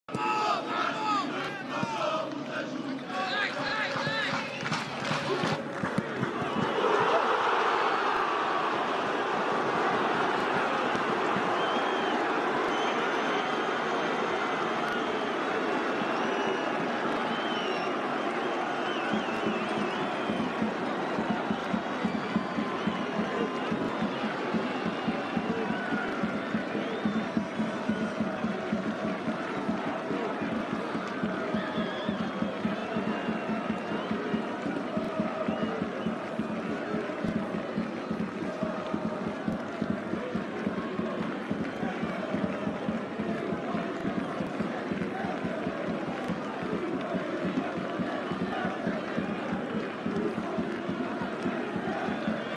Goal | Golo Paulinho: Portimonense 0-(1) Sporting (Liga 22/23 #23)
Excelente movimentação de Paulinho, o passe foi de Pedro Gonçalves, já em queda o avançado leonino atira de primeira e desbloqueia o marcador!